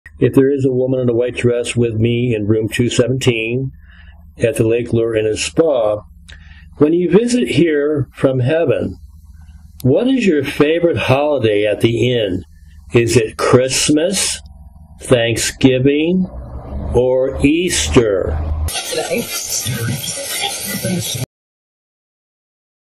Likes Christmas
Welcome to edwards paranormal. Read message while you listen. Message is "Likes Christmas.“. Audio from a spirit woman seen by guests wearing a white dress. Audio captured in room 217 at the 1927 Lake Lure Inn and Spa on a Steve Hultay spirit box Reference: NCPA Investigation February 25, 26